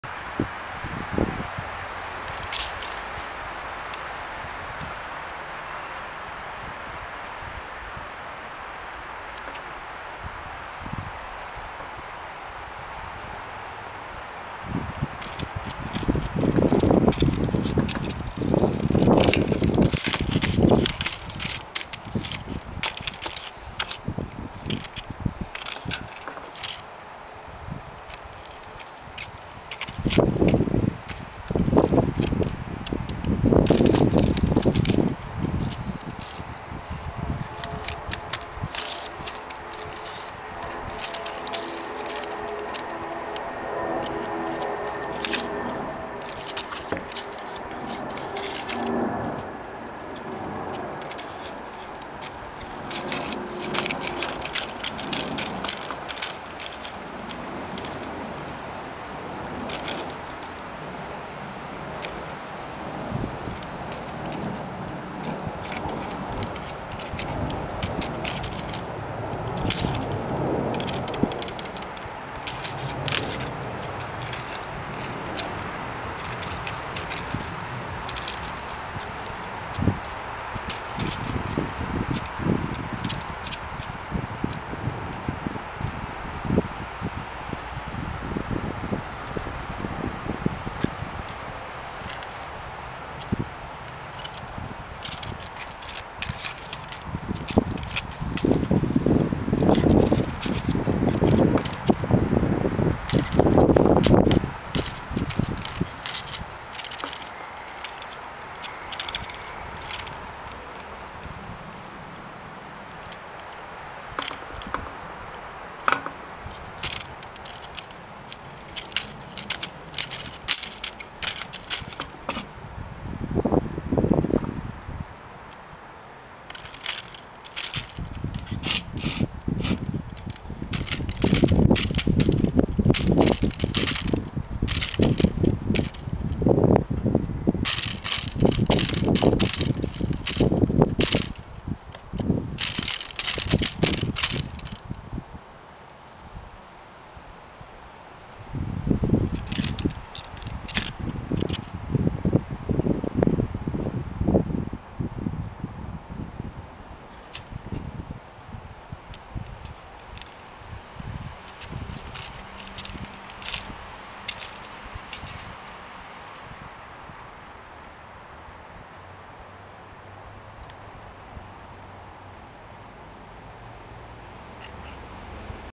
Cat Beating a Pinwheel.
Pepper hitting a pinwheel.